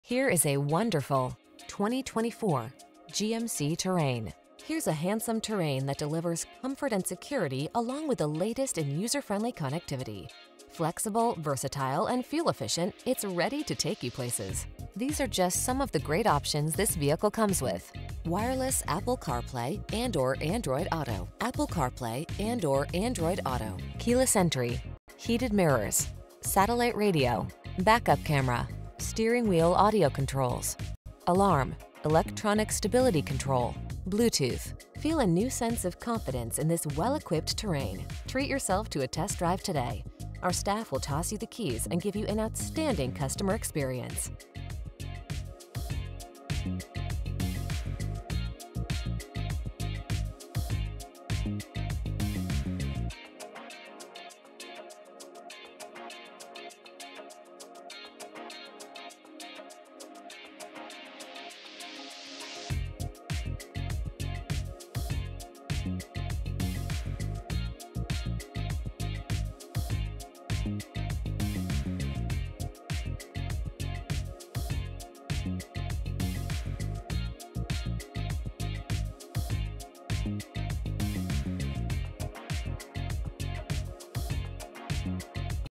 2024 GMC Terrain SLE Naperville, Plainfield, Aurora, Oswego, Chicago IL
New 2024 GMC Terrain SLE available at Woody Buick located in 1585 W Ogden Ave, Naperville, IL, 60540.
Stock#: G12599 - VIN#: 3GKALMEG0RL378025

Here is a wonderful   2024  GMC Terrain. Here's a handsome Terrain that delivers comfort and security along with the latest in user-friendly connectivity. Flexible, versatile, and fuel-efficient, it's ready to take you places! These are just some of the great options this vehicle comes with: Wireless Apple Carplay®/Android Auto®, Apple Carplay®/Android Auto®, Keyless Entry, Heated Mirrors, Satellite Radio, Back-Up Camera, Steering Wheel Audio Controls, Alarm, Electronic Stability Control, Bluetooth®. Feel a new sense of confidence in this well-equipped Terrain. Treat yourself to a test drive today. Our staff will toss you the keys and give you an outstanding customer experience.